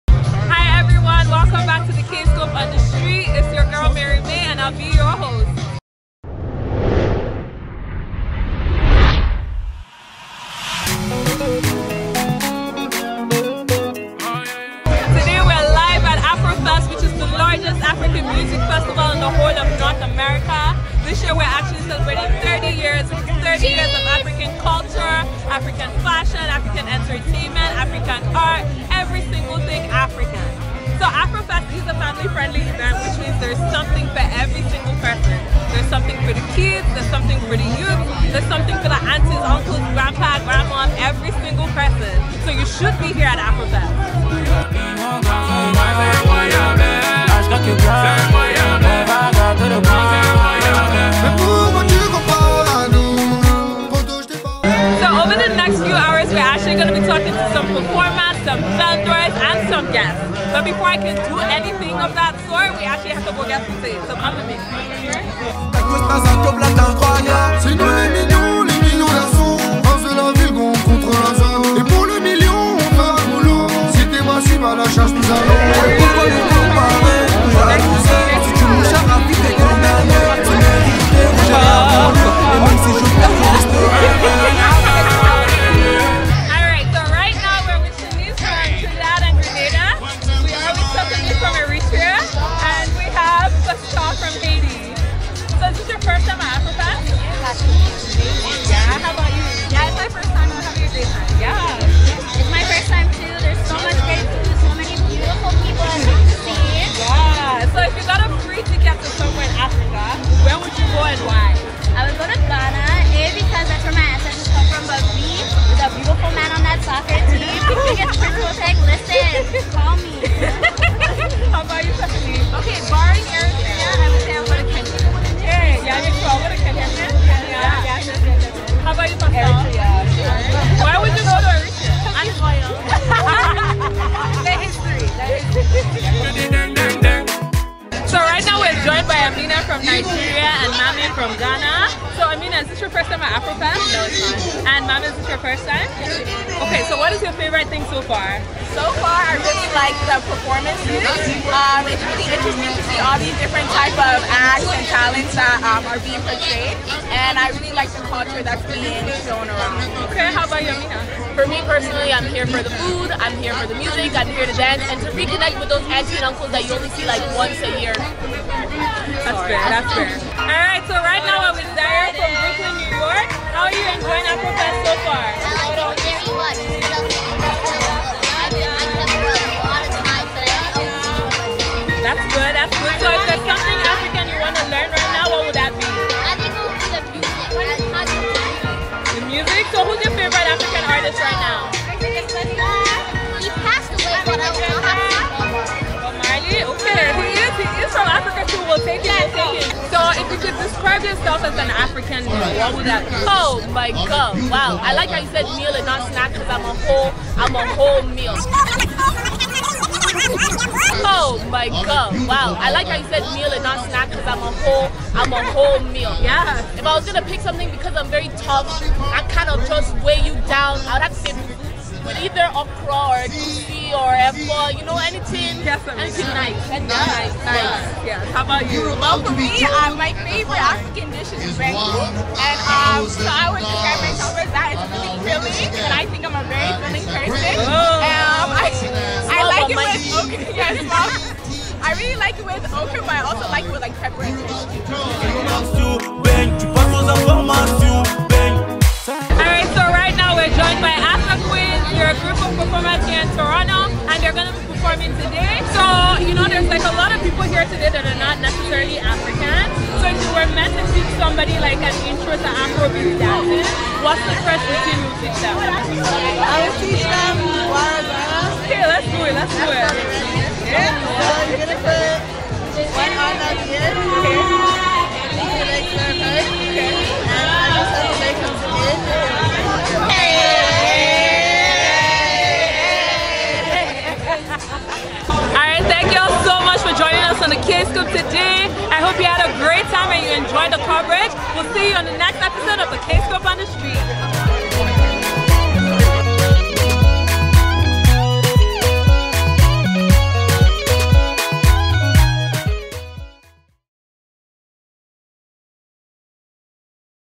Afrofest 2018! The Kscope On The Street
It's your fave gal Mayree May! On the streets of Toronto live at Afrofest Toronto! Afrofest Toronto is the Largest African music festival in all of North America and it is hosted by Music Africa.